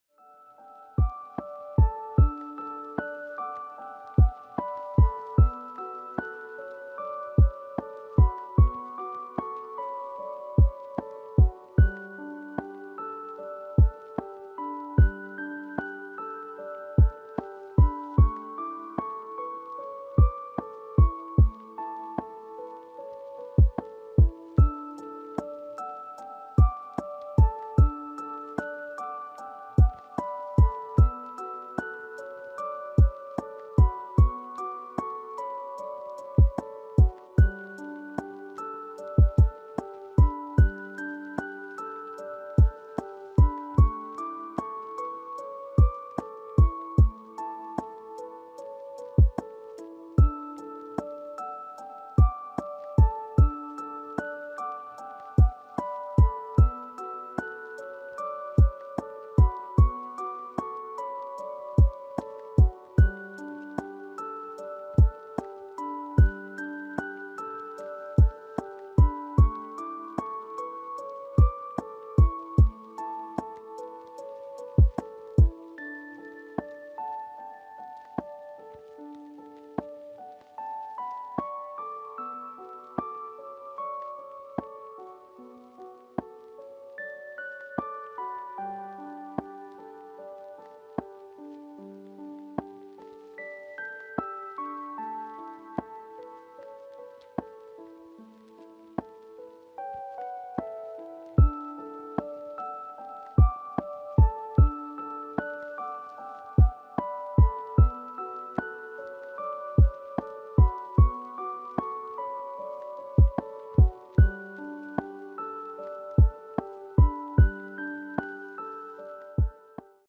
Short Christmas WRCP/WRB Compilation
The stages are numbered differently than the KSF version of the map. Also neither of the Christmas maps were grinded and some of the stages were bugged so this is all we have for the festivities. Next year it will be better.

(102t)